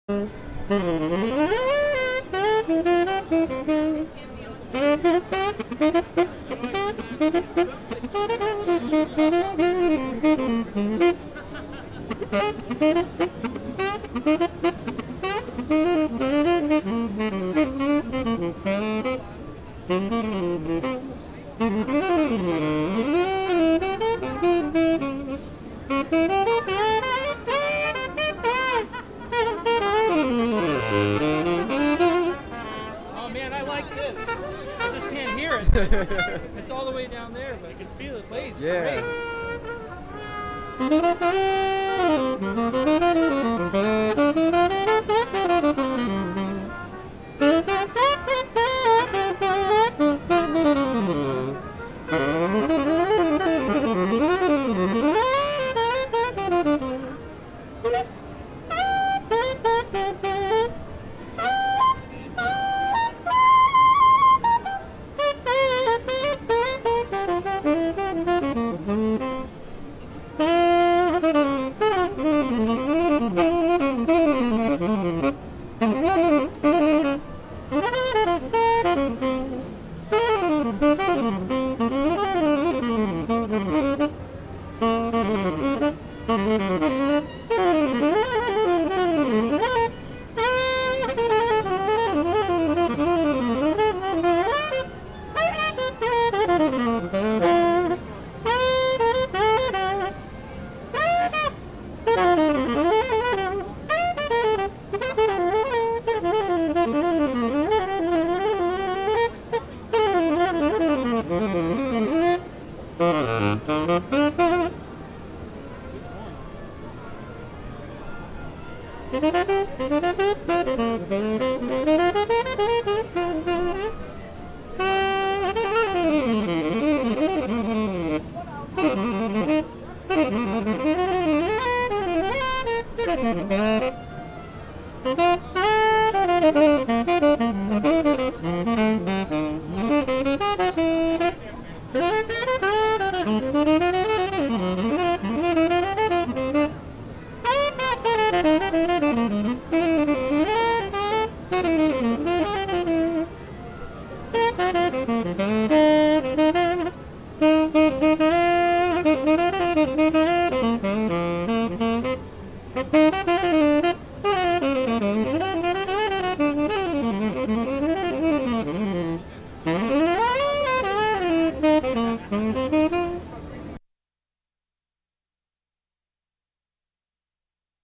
Richard Elliot
I caught Richard Elliot play a straight tenor. I heard some old Tower of Power licks from across the covention hall and I knew exactly who it was so I had to stop by.